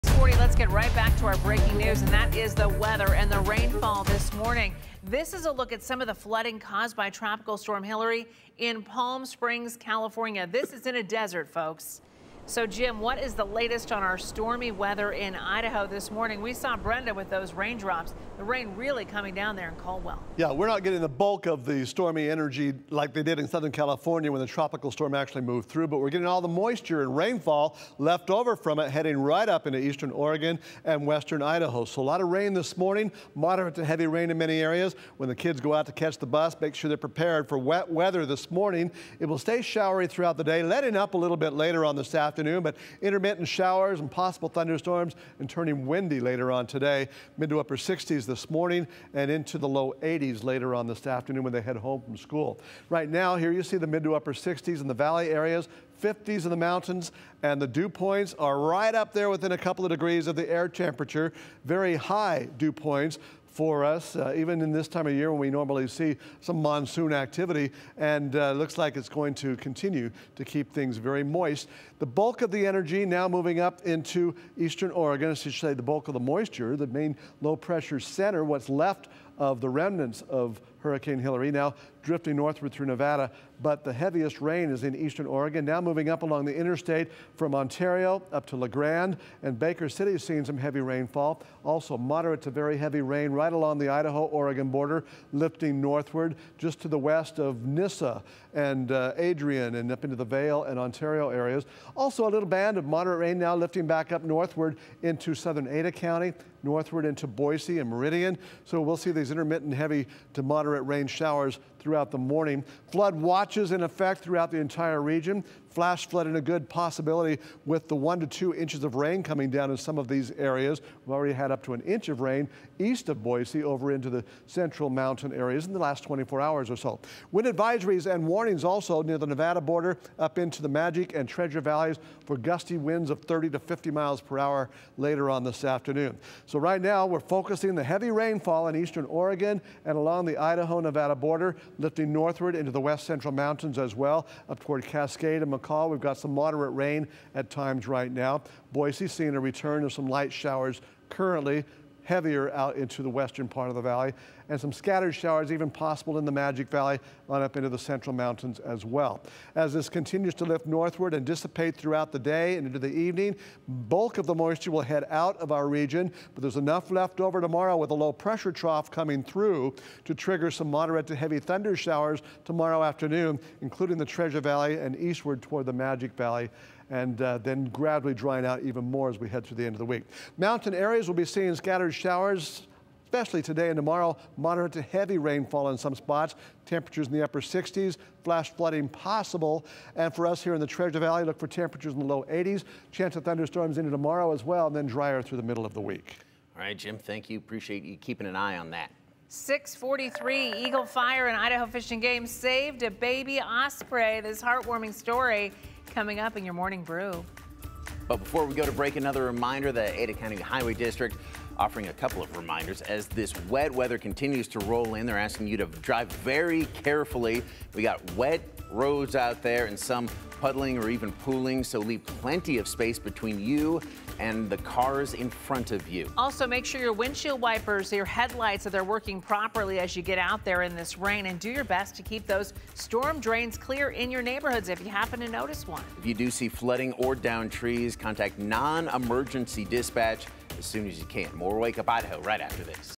Remnants of Hurricane Hilary will bring periods of moderate to heavy rain & t-storms with localized
The daily forecast from Idaho's largest, most experienced team of meteorologists.